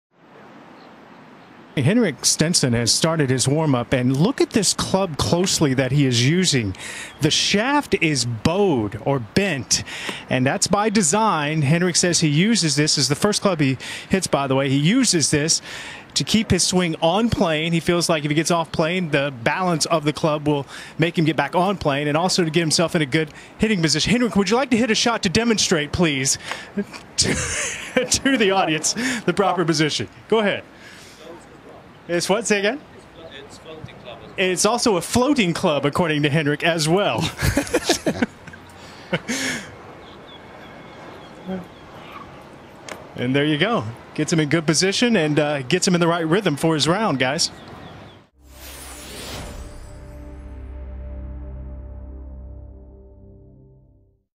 Henrik Stenson uses bowed club to warm up at Cadillac Championship
In the final round of the 2015 World Golf Championships - Cadillac Championship, Henrik Stenson warms up with a bowed club on the range to get into a good rhythm. .